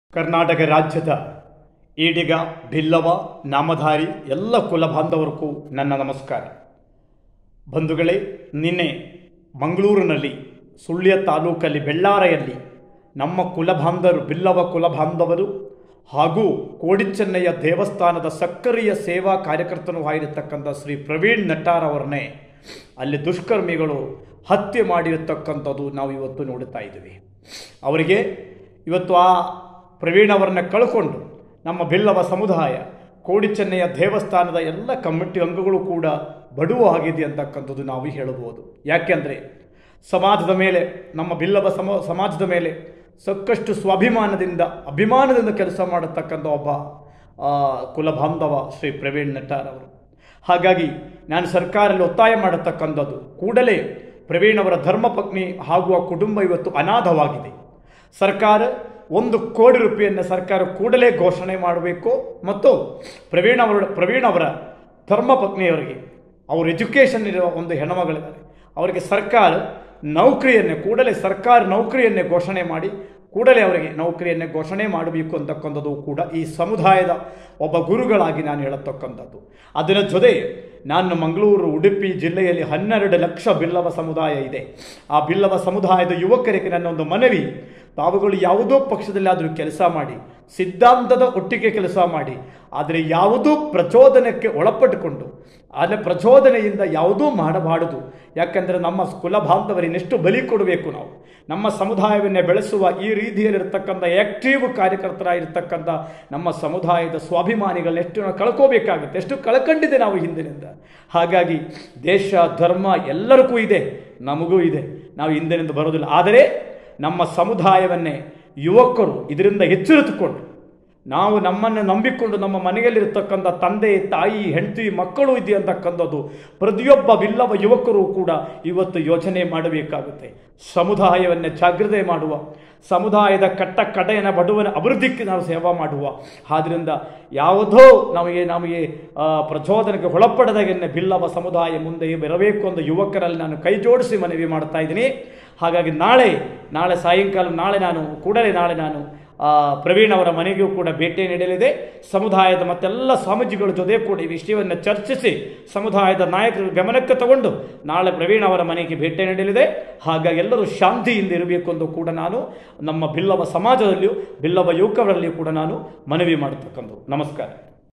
"ರಾಜ್ಯ ಸರ್ಕಾರ ಒಂದು ಕೋಟಿ ರೂಪಾಯಿ ಹಾಗೂ ಅವರ ಧರ್ಮಪತ್ನಿಗೆ ಸರ್ಕಾರಿ ನೌಕರಿಯನ್ನು ಘೋಷಿಸಬೇಕು.." | SkyNET Kannada
ಪ್ರವೀಣ್ ಅವರ ಕುಟುಂಬಕ್ಕೆ ಕೂಡಲೇ  ರಾಜ್ಯ ಸರ್ಕಾರ ಒಂದು ಕೋಟಿ ರೂಪಾಯಿ ಪರಿಹಾರ  ಹಾಗೂ ಅವರ ಧರ್ಮಪತ್ನಿಗೆ ಸರ್ಕಾರಿ ನೌಕರಿಯನ್ನು ಘೋಷಿಸಬೇಕು ಆರ್ಯ ಈಡೀಗ ರಾಷ್ಟ್ರೀಯ ಮಹಾಮಂಡಲದ ಅಧ್ಯಕ್ಷ ಡಾ. ಪ್ರಣವಾನಂದ ಸ್ವಾಮೀಜಿ ಆಗ್ರಹಿಸಿದರು.